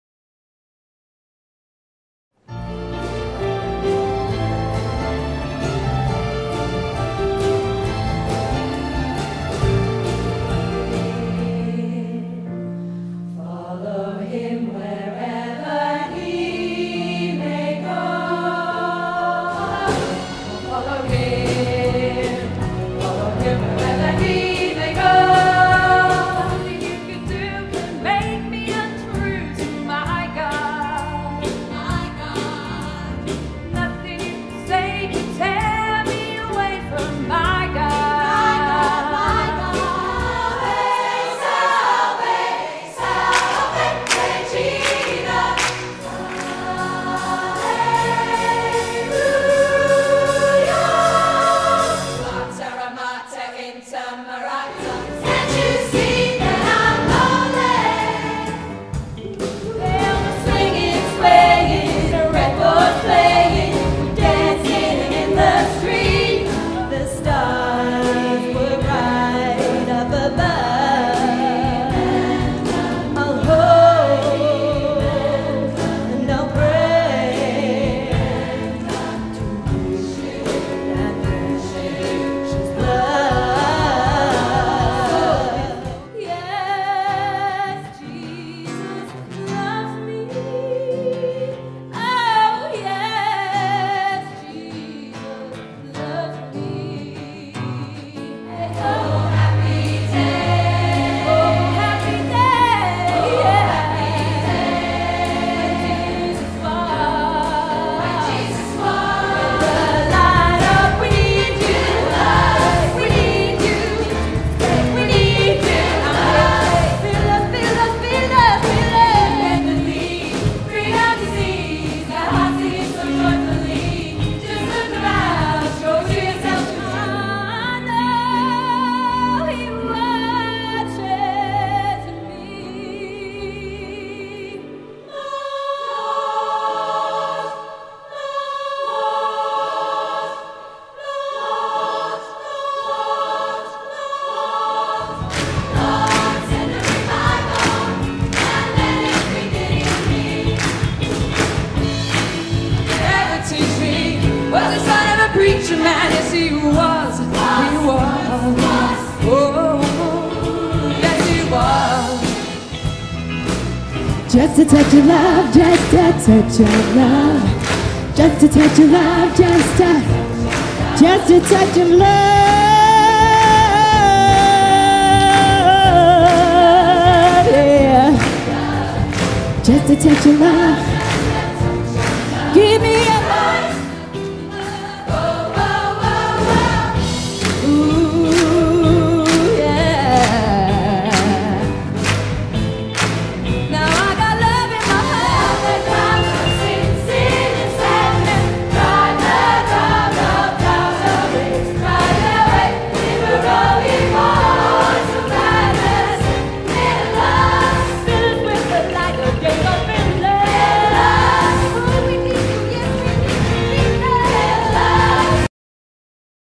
Back In The Habit Manchester Show Choir
4 years ago398 views
Manchester Show Choir 2011 "Back In The Habit" filmed at The Plaza in Stockport by Handel
FULL SHOW IS AVAILABLE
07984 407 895